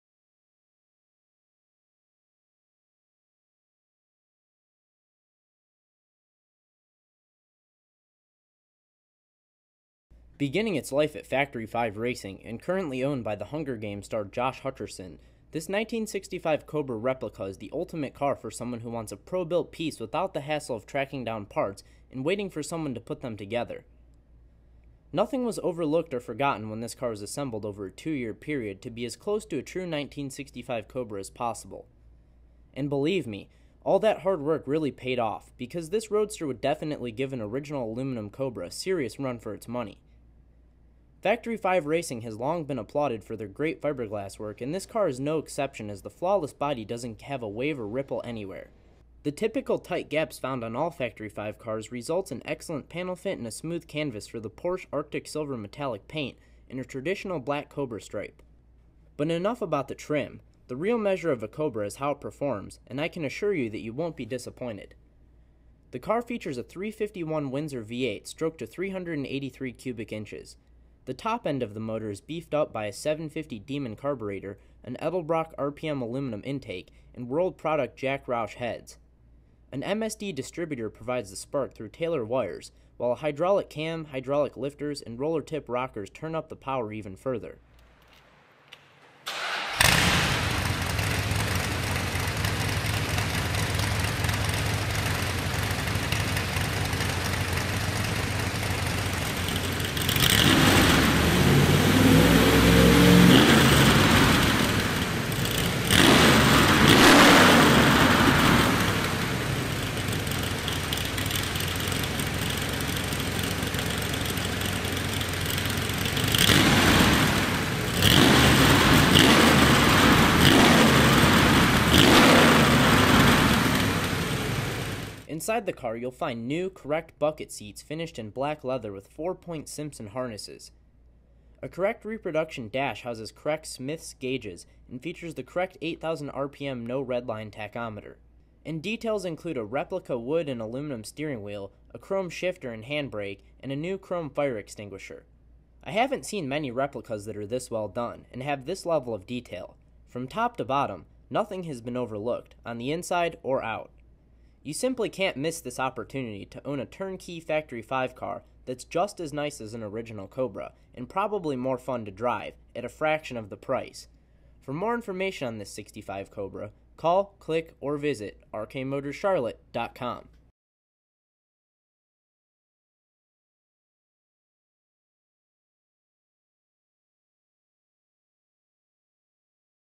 132993 / 1965 Shelby Cobra
Beginning its life at Factory Five Racing, and currently owned by The Hunger Games star Josh Hutcherson, this 1965 Cobra replica is the ultimate car for someone who wants a pro-built piece without the hassle of tracking down parts and waiting for someone to put them together!

Nothing was overlooked or forgotten when this car was assembled over a two year period to be as close to a true 1965 Cobra as possible.  And believe me, all that hard work really paid off because this roadster would definitely give an original aluminum Cobra a serious run for its money. Factory Five has long been applauded for their great fiberglass work and this car is no exception as the flawless body doesn't have a wave or ripple anywhere. The typical tight gaps found on all Factory Five cars results in excellent panel fit and a smooth canvas for Porsche Artic Silver Metallic and a traditional black Cobra stripe.

But enough about the trim, the real measure of a Cobra is how it performs and I can assure that you won't be disappointed. The car features a 351 Windsor V8 stroked to 383 cubic inches. The top end of the motor is beefed up by a 750 Demon carburetor, an Edelbrock RPM aluminum intake and World Product Jack Roush heads. An MSD Distributor provides the spark through Taylor wires while a hydraulic cam, hydraulic lifters and roller tip rockers turn up the power even further. Cooling isn't an issue as a Ron Davis Racing aluminum radiator and an aluminum water pump keep everything in check. Under the hood a Cobra air filter and Cobra valve covers inversely complement the black on silver exterior paint. The clean, flat black engine bay is completed by aluminum pulleys and braided hoses adding just the right amount of detail and bright work. A look underneath the car shows the continuation of that clean black and metal theme with the black frame and suspension components contrasting the natural finish metal floorpan.

One of the best things about buying a complete car is the modern technology already incorporated into that car. Bolted into this Cobra's correct frame is a modern TKO 5 speed performance transmission. That transmission feeds power to a proven Mustang based 8.8 inch rear axle housing with 3.73 gears. When this drivetrain is combined with Factory Five's modern Mustang 5.0 based suspension, the result is a car that's insanely fun to drive because of its low power to weight ratio but doesn't need to stop at every gas station in the process. But if you do need to stop, it shouldn't be a problem with power assisted front disc brakes.

Inside the car you'll find new correct bucket seats finished in black leather with four point Simpson harnesses. A correct reproduction dash houses correct Smiths gauges and features the correct 8,000 RPM no redline tachometer. New carpet is protected by Cobra floor mats. And details include a replica wood and aluminum steering wheel, a chrome shifter and hand brake and a new chrome fire extinguisher. Behind the cockpit a fully carpeted trunk houses only the battery and retains much of its usability. I haven't seen many replicas that are this well done and have this level of detail. From top to bottom, nothing has been overlooked inside or out.

You simply can't miss this opportunity to own a turn-key Factory Five car that's just as nice as an original Cobra, and probably more fun to drive, at a fraction of the price! for more information on this awesome car!